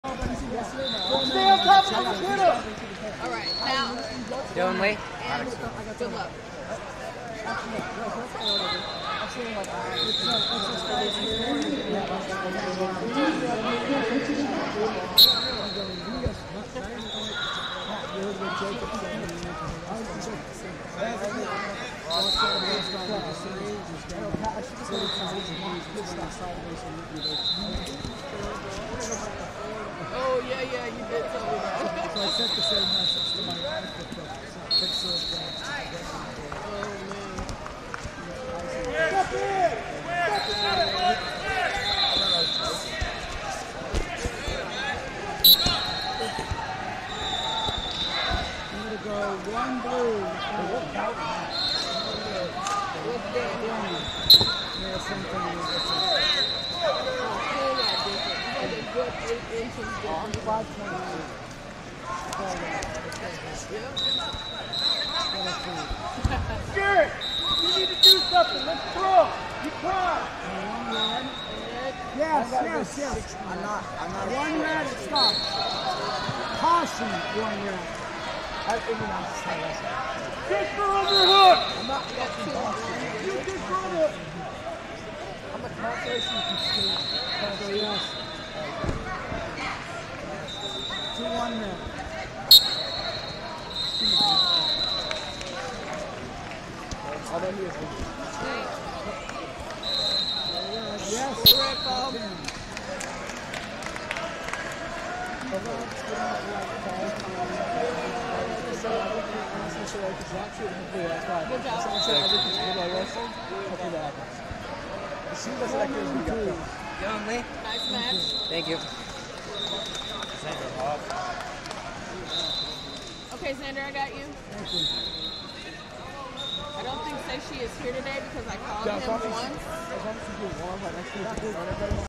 145 Lbs Cons. Round 2 - Dillon Le, Newberg High School Wrestling Vs Matix Cooklin, Forest Grove Hi
Flosports-partner-access/2023 Northwest 16U & Junior Regional Championships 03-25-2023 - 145 Lbs Cons. Round 2 - Dillon Le, Newberg High School Wrestling Vs Matix Cooklin, Forest Grove High School Wrestling Bdec